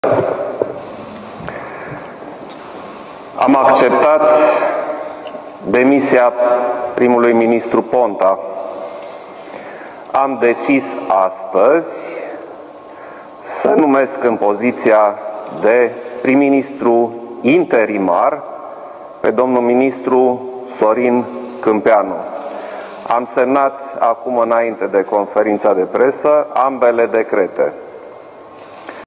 Declarație de presă a președintelui României, Klaus Iohannis
Sorin Câmpeanu a fost desemnat prim-ministru interimar!
Titlul evenimentului: Declarație de presă a președintelui României, Klaus Iohannis